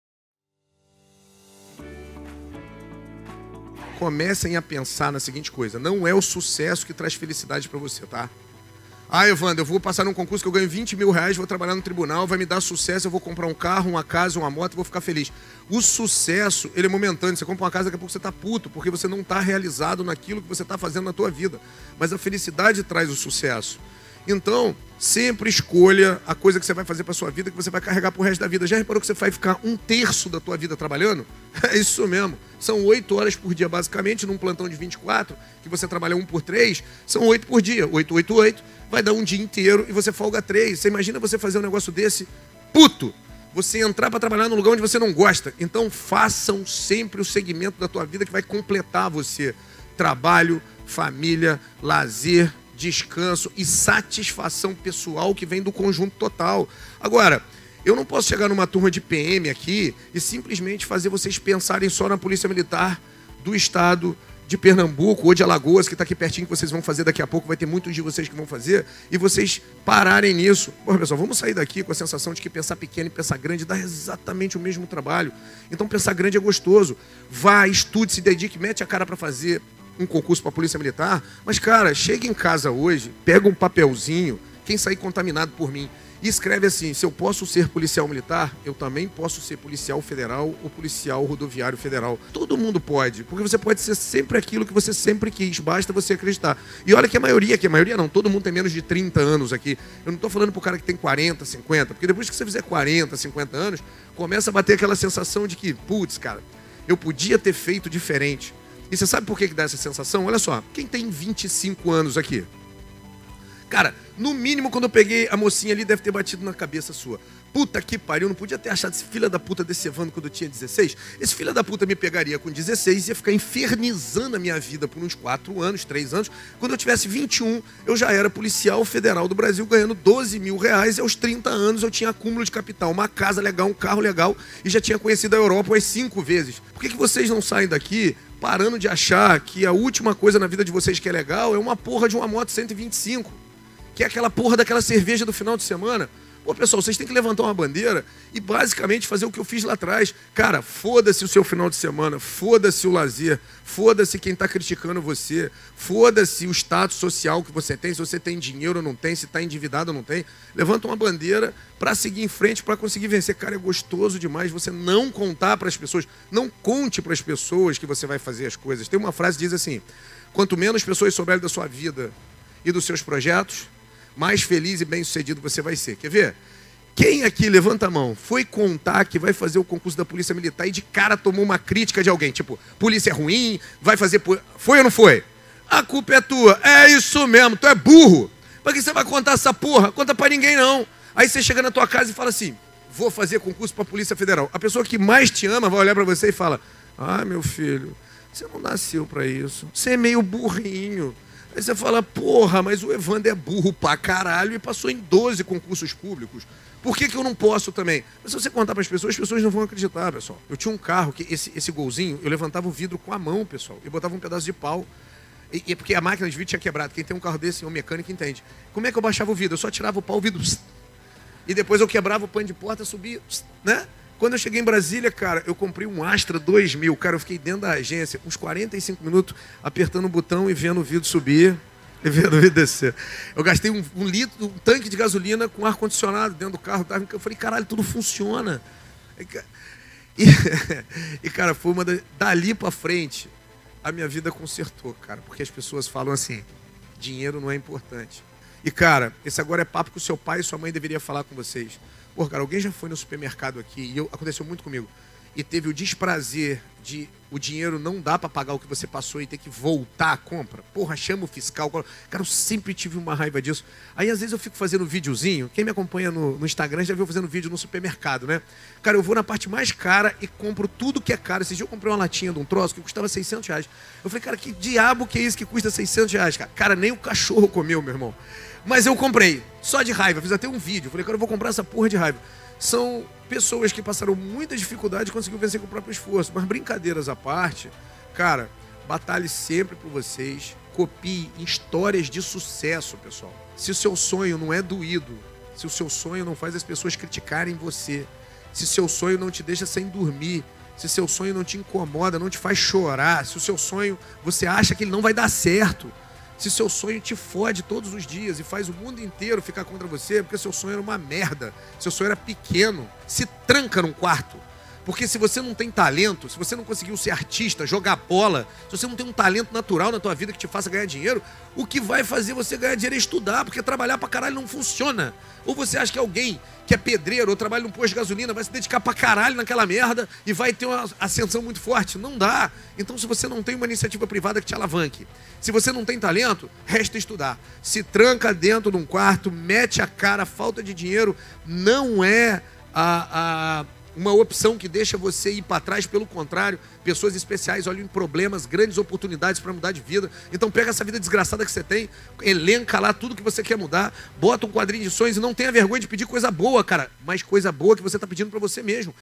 NÃO PASSE SUA VIDA FAZENDO ALGO QUE TE DEIXA PUT*! Evandro Guedes - Fábrica de Valores
Todo sucesso é momentâneo quando você não está realizado com o que faz.
Batalhe para se realizar profissionalmente e ser feliz todos os dias de sua vida com a satisfação pessoal de fazer o que ama!